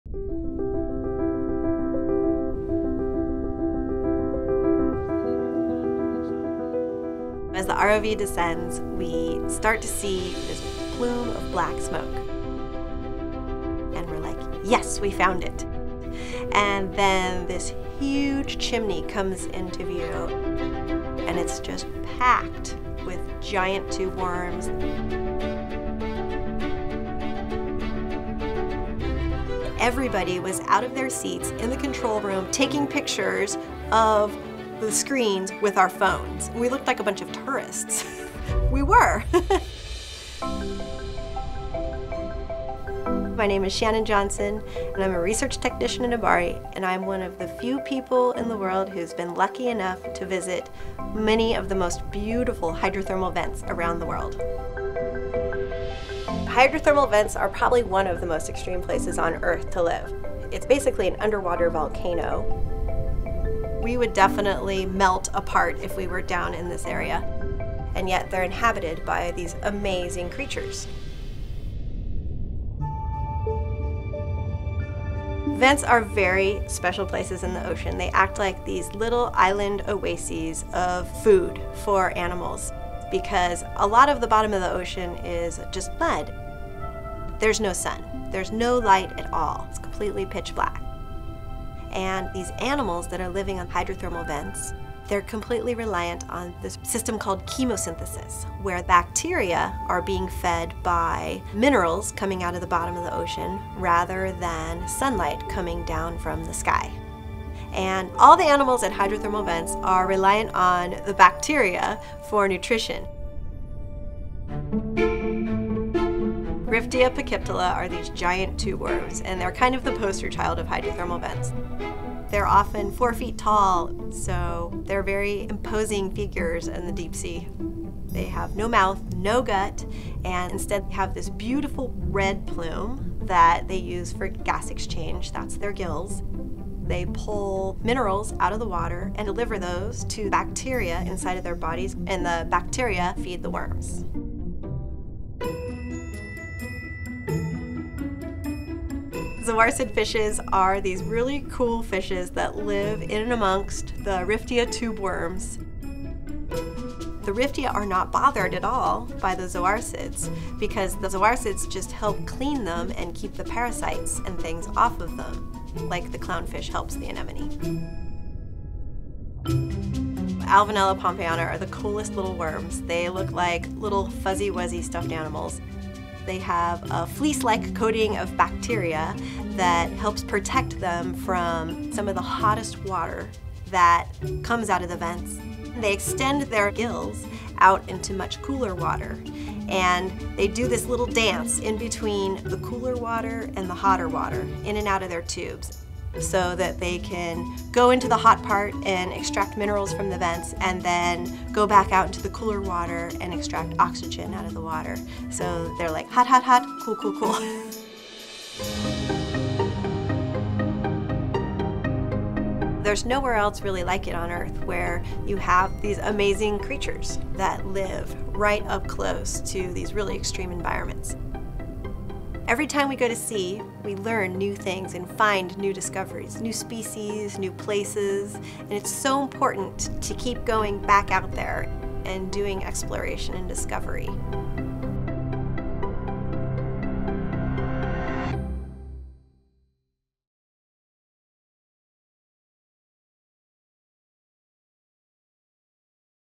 Hydrothermal vents: Explore a bizarre deep ocean habitat
As the Monterey Bay Aquarium prepares for the launch of the 2022 exhibit, "Into the Deep: Exploring Our Undiscovered Ocean," MBARI deep-sea biologist Shannon Johnson shares some of her favorite stories about hydrothermal vents and the life that thrives at these magnificent deep-sea habitats. Towering chimneys spew hot, mineral-laden water out of the deep seafloor. The astonishing communities that live on and around the vents have evolved to not only withstand but flourish in the extreme temperature and chemical conditions.

Colonies of giant tubeworms, Riftia pachyptila, have a symbiotic relationship with chemosynthetic microbes that convert methane and sulfides from the water into energy for the worms. Bizzare-looking zoarcid vent fish, Thermarces cerberus, live among the tubeworms, feeding on parasites off their tubes. Small Pompeii worms, Alvinella pompejana—the most heat-tolerant animals on the planet—extract minerals out of the scalding vent fluid.

Our ocean is the largest home for life on Earth. For more than three decades, MBARI researchers have been exploring the deep ocean with remotely operated vehicles, discovering fascinating ways that creatures have evolved to live in these harsh environments. Now, with more companies looking to extract resources from unique areas like hydrothermal vents, it is more important than ever to study the deep sea and the wonders it holds. There is much to learn in order to manage ocean resources wisely. If we are not careful, there is much to lose before we even know what may be lost.

Learn more about MBARI’s research and the incredible discoveries we've made as we explore the ocean

Monterey Bay Aquarium video production team: Christy Chamberlain, Presley Adamson
Editor: Lou Laprocido
MBARI video production team: Kyra Schlining, Susan von Thun, Nancy Jacobsen Stout, Heidi Cullen